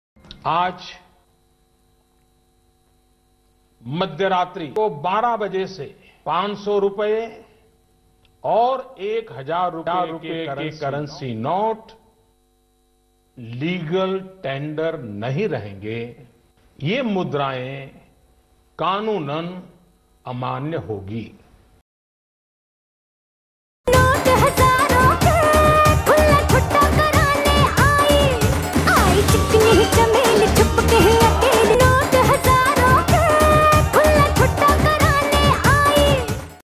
WOMEN'S REACTION Rs 500 and 1000 currency notes to be void from midnight
WOMEN'S REACTION After watching PM Modi speech to declares Rs 500 and 1000 currency notes to be void from midnight